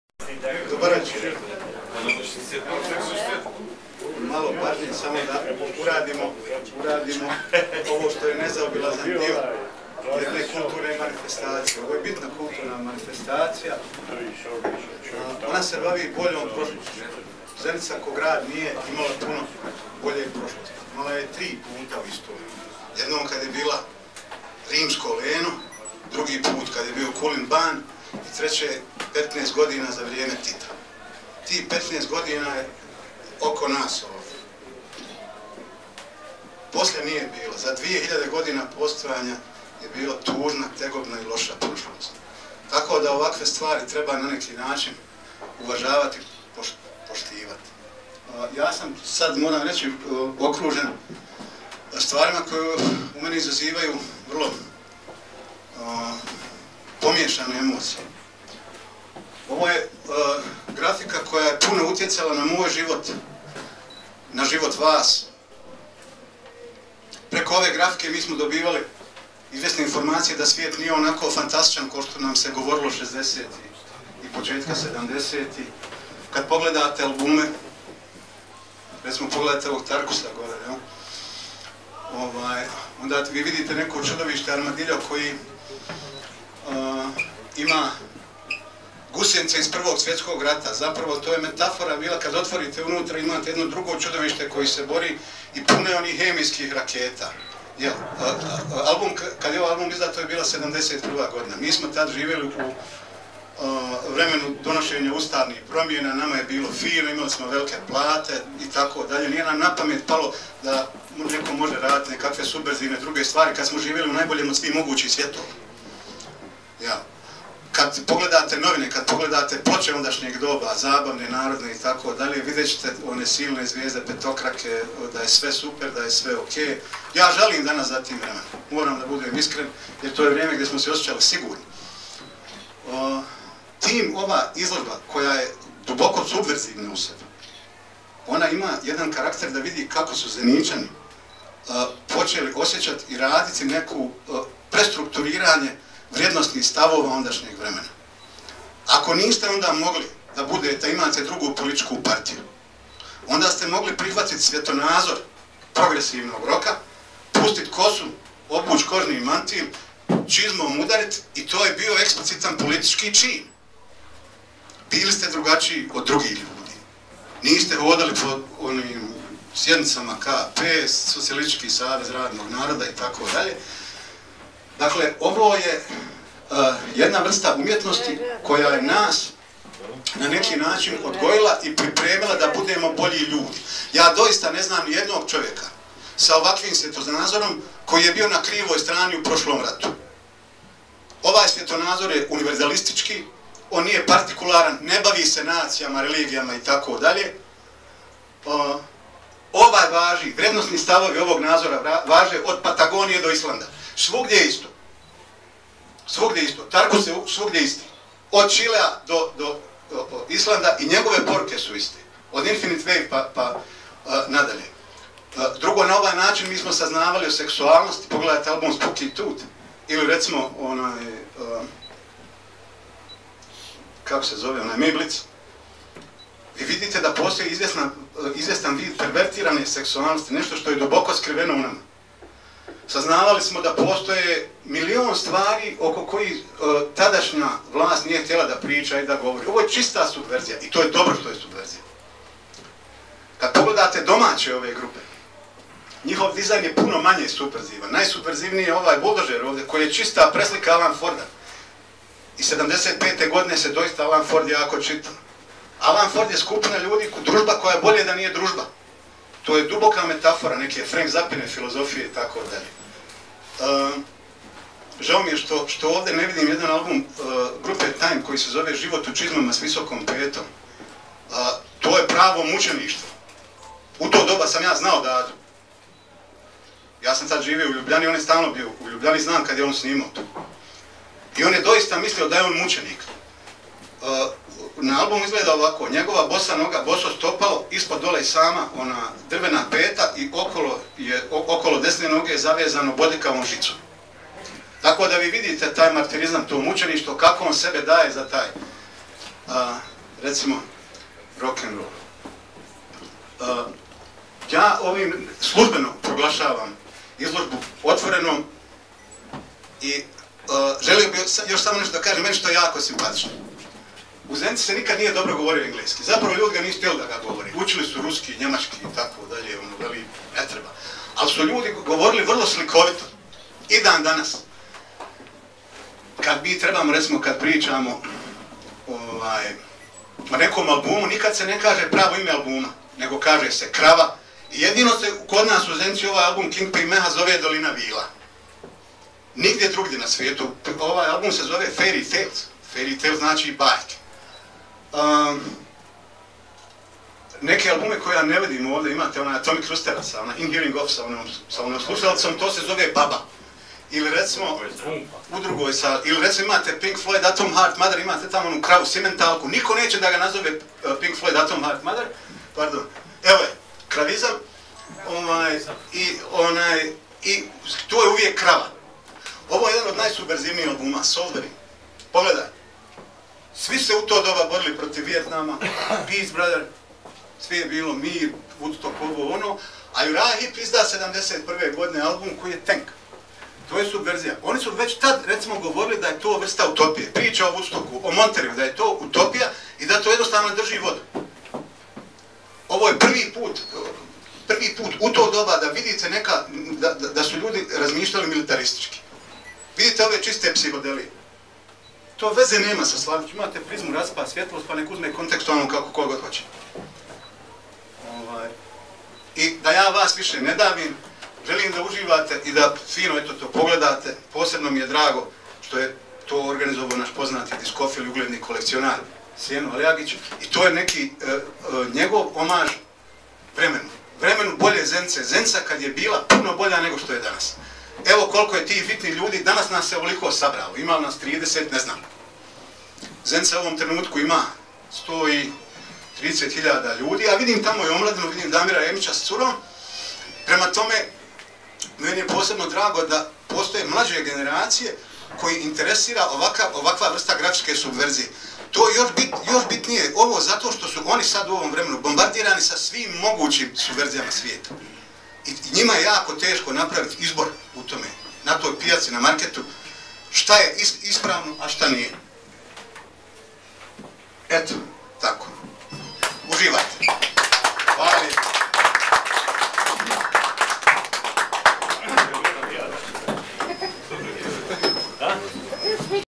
sead pasic, govor na otvaranju izlozbe omota gramofonskih ploca - 04.06.10_xvid.avi
U Rock Club Hey Joe Zenica je u petak 04,06 2010 godine otvorena jizlozba omota gramofonskih ploca progresivnog rock -a od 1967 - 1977, uglednog kolekcionara i diskofila Senada Alijagica. Izlozbu je otvorio Sead Pasic